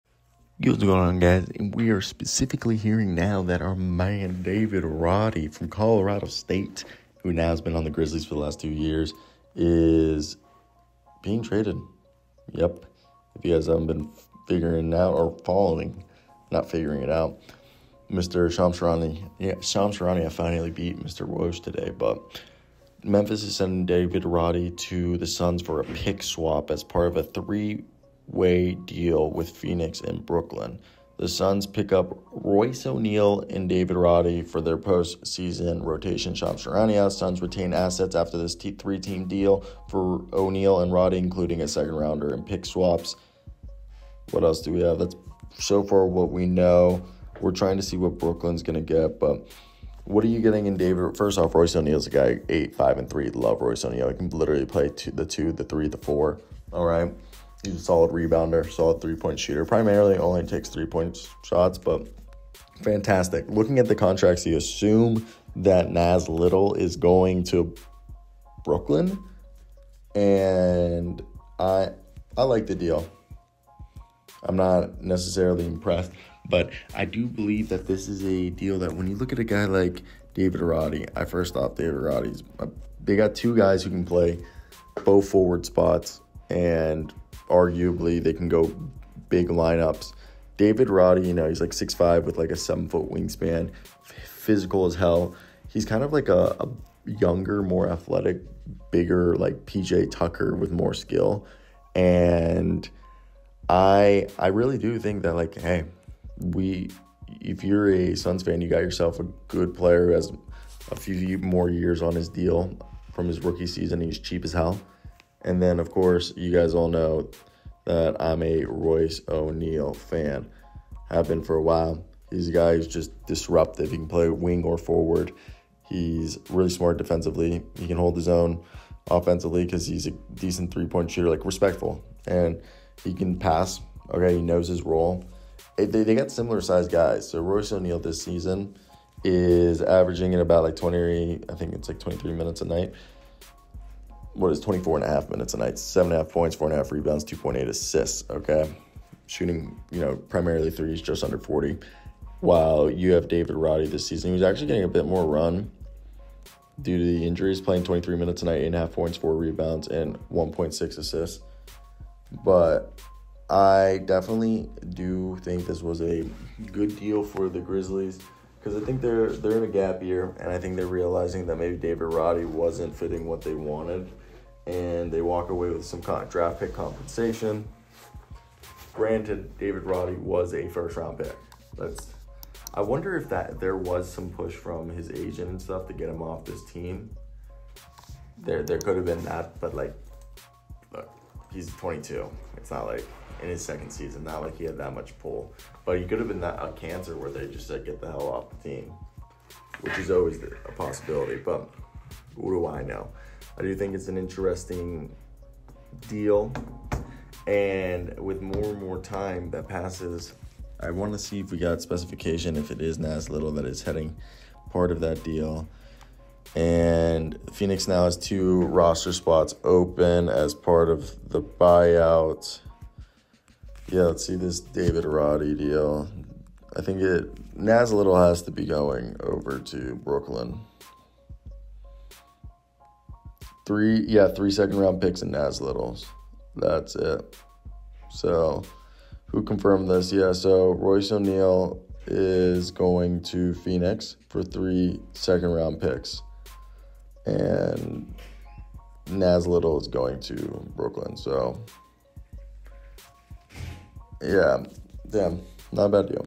Bucks Trade For Patrick Beverley, Cam Payne, Second Round Pick
The Milwaukee Bucks have agreed to acquire Patrick Beverley from the Philadelphia 76ers for Cameron Payne and a second round pick in 2027.

Beverley will give the Bucks an upgrade defensively in the backcourt.

The Bucks also had interest in Kris Dunn and Naji Marshall.

Adrian Wojnarowski/ESPN